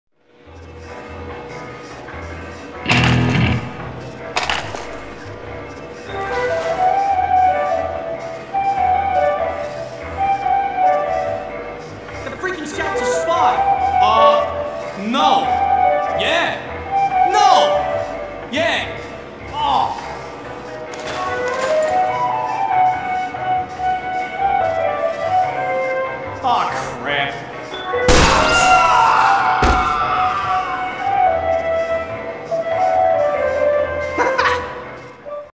(SFM) - That Scout Is A Spy!
Dont You Get Mad When Your Teammates Spam The Spy Voice On You?? XD